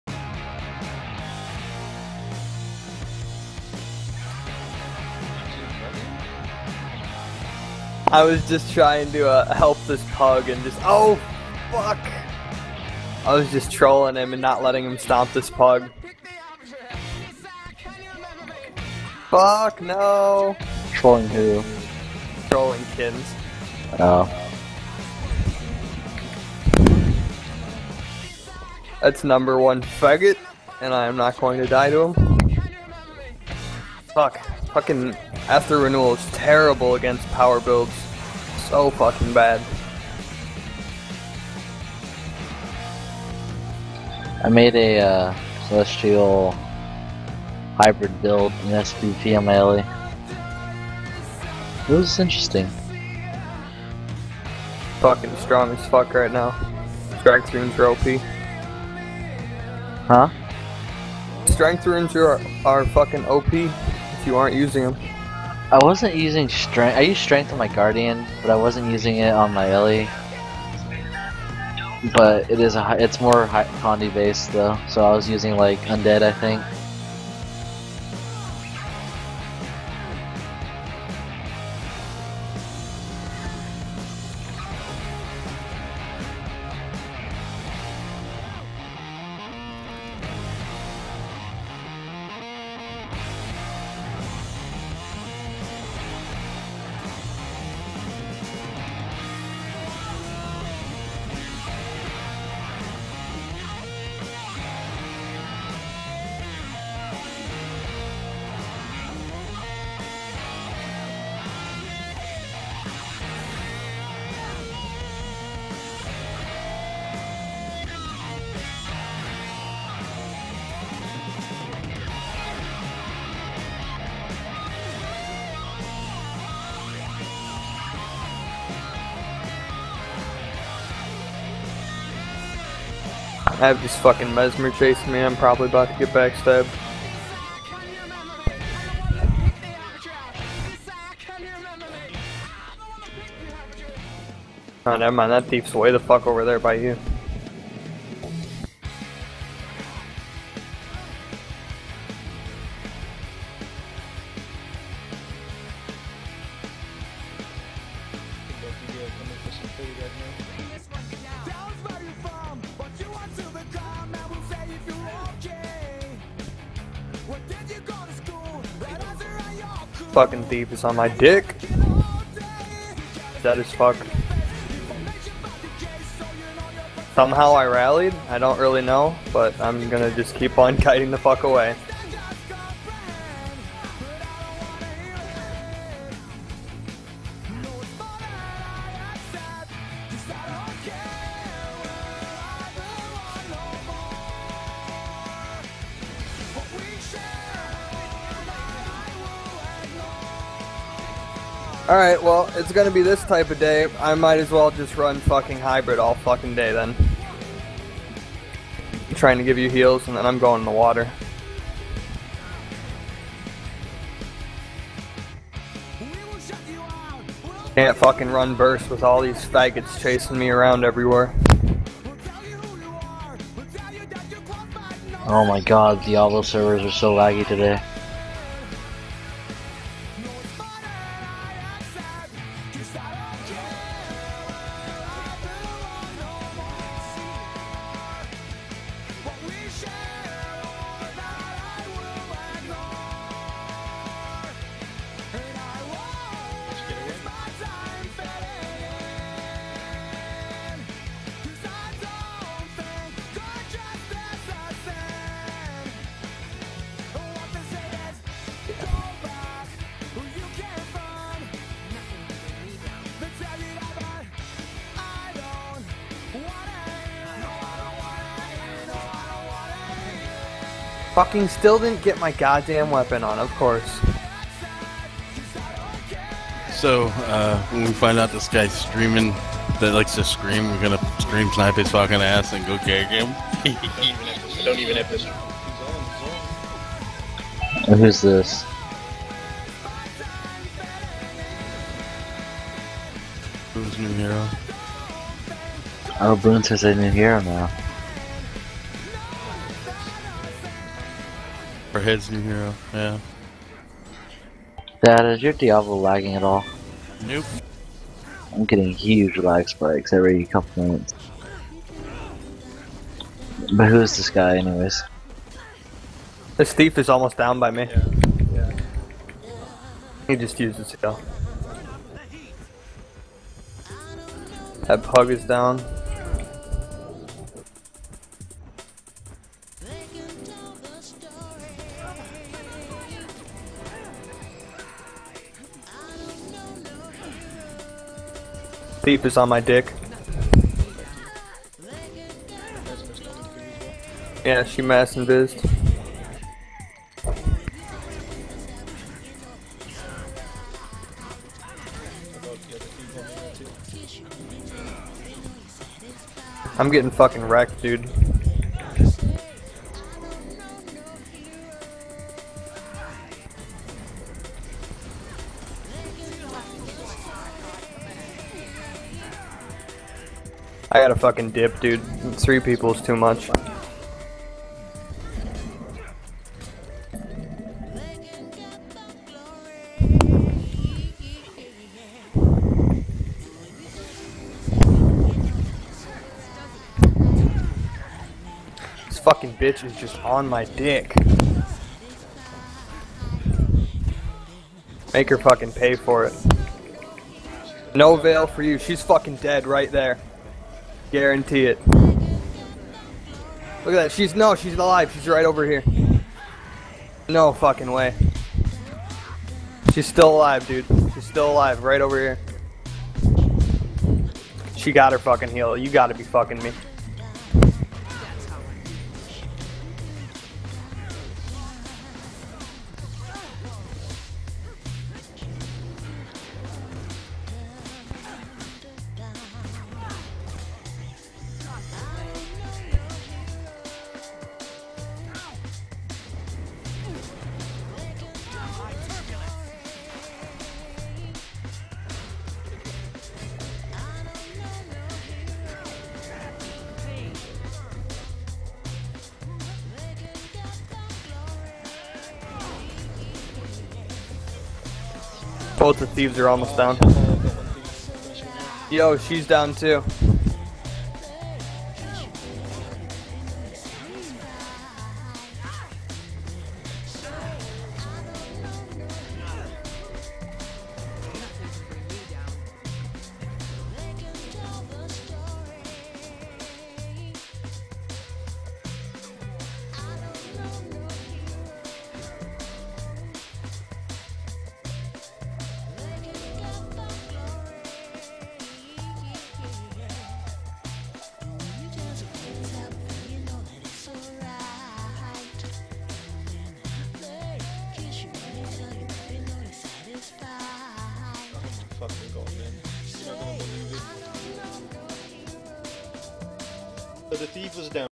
[Twitch] Episode 143 - Skirmish with Hedrokka
A nice little skirmish with Hedrokka. Couple of Aunz players with a couple of NS players. We win the fight and then this guardian charges in with his aoe shit and just keeps rezzing the fucking thief... GTFO!

Tough fight for Hed, 2 thieves and a mesmer can be extremely rough on an ele.